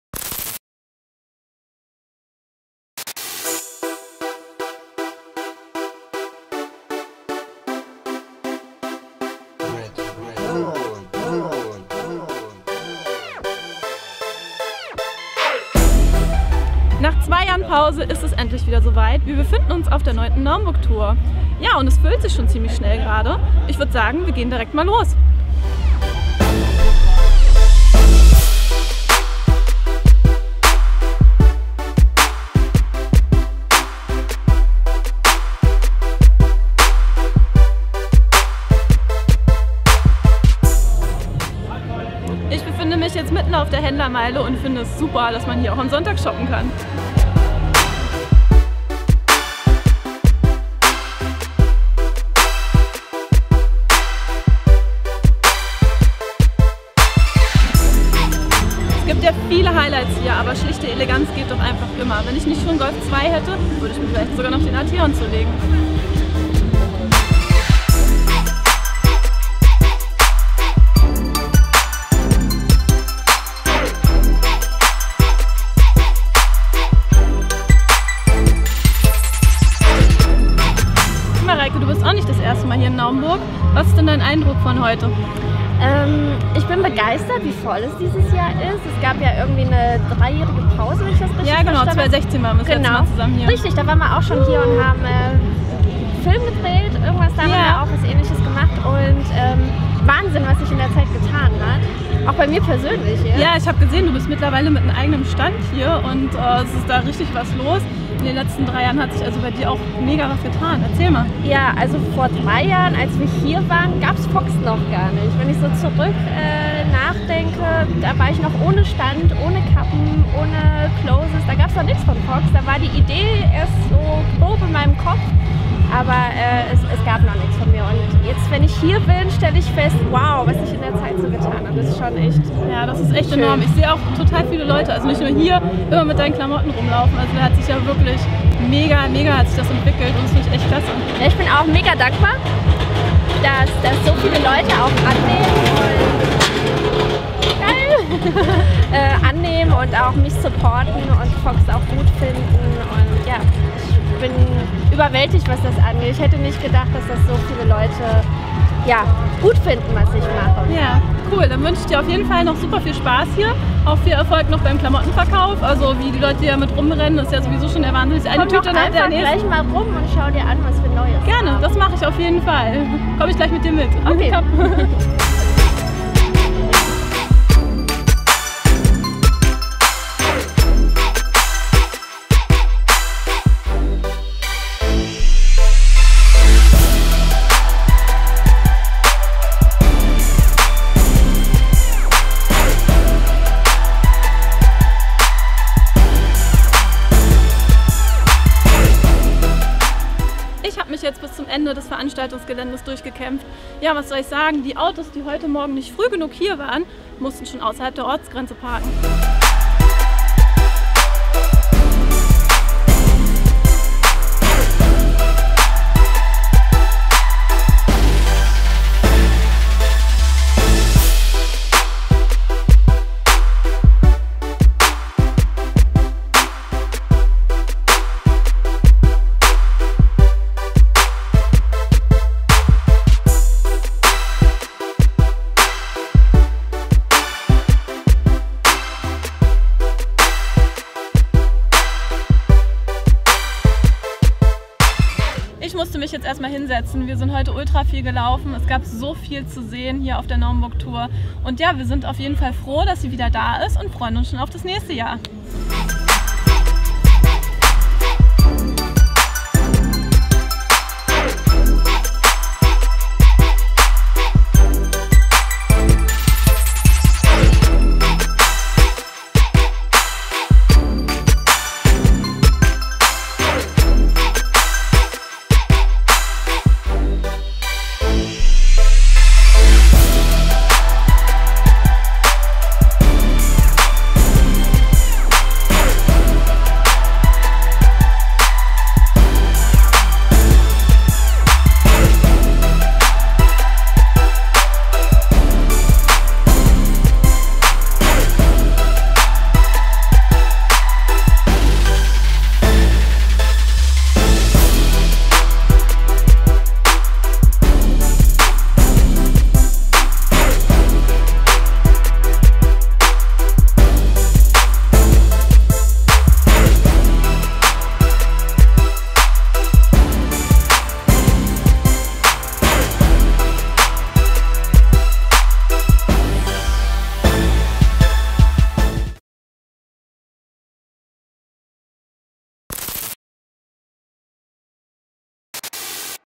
Naumburg Tour 2019
Die Naumburg Tour ist zurück und Ricarda Bongartz sieht sich das Ganze live und in Farbe an. Dabei trifft sie unter anderem auf Szeneliebling Mareike Fox.

Soundtrack: RedHood Beatz